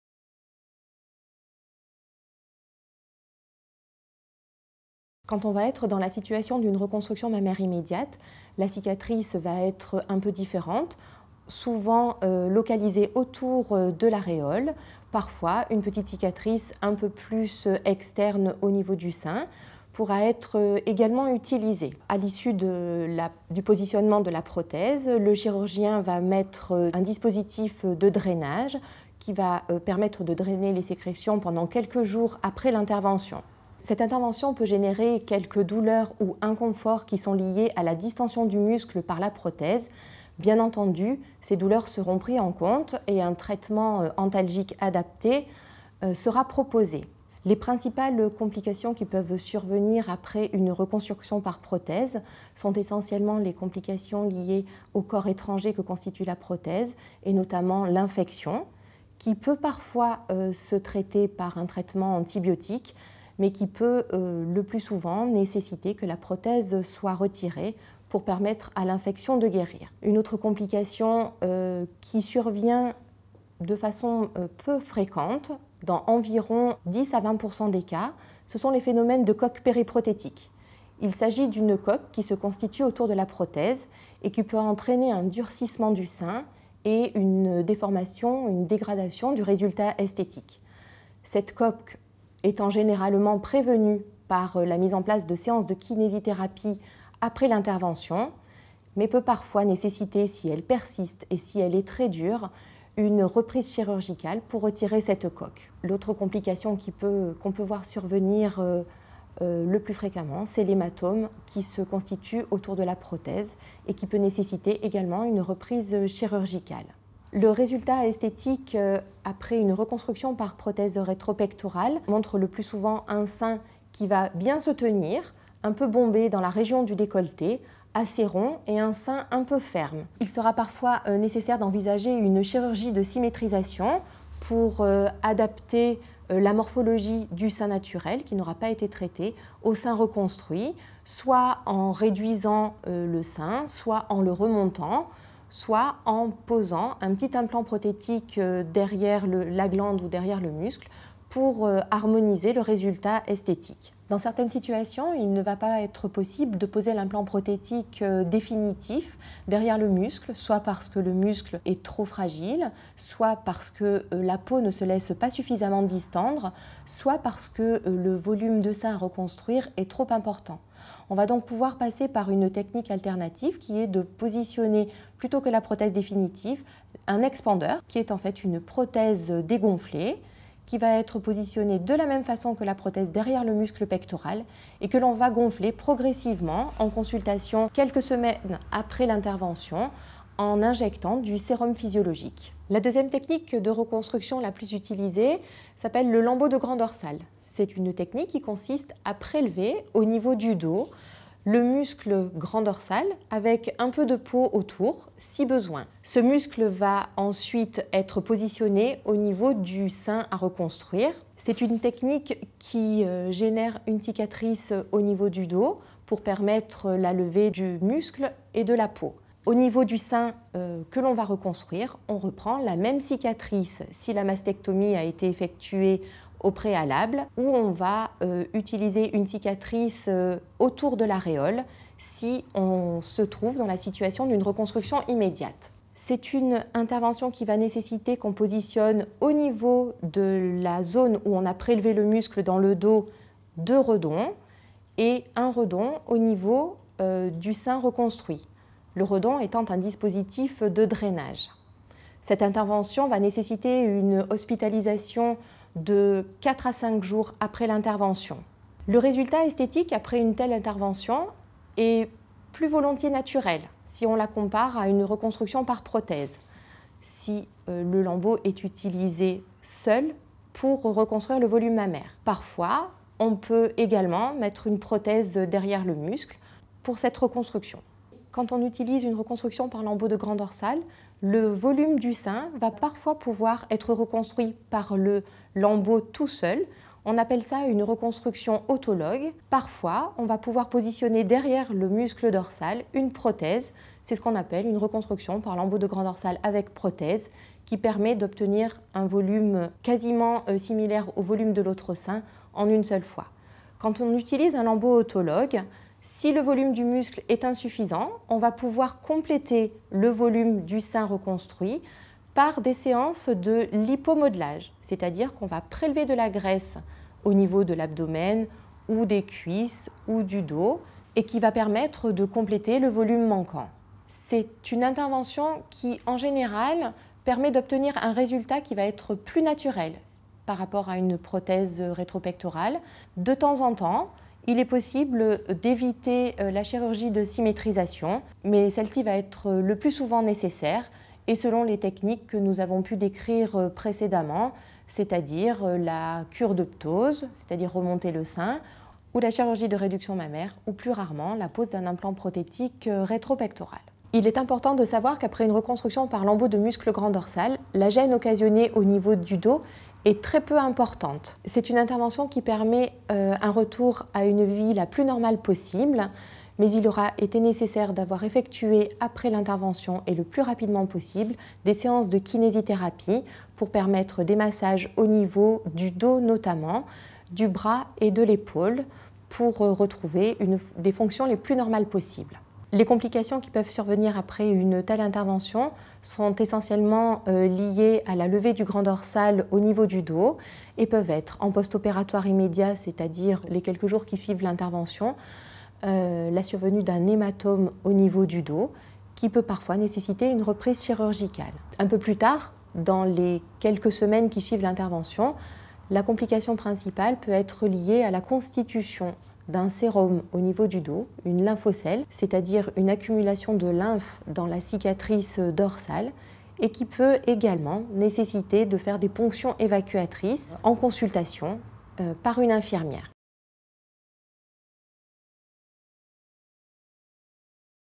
Information sur les techniques de reconstruction mammaire au Centre Antoine Lacassagne
Vidéo à destination des patientes sur les reconstructions mammaires en chirurgie sénologique par Dr Magali Dejode, chirurgien sénologue au sein du Pôle de Chirurgie oncologique générale, gynécologique et sénologique du Centre Antoine Lacassagne.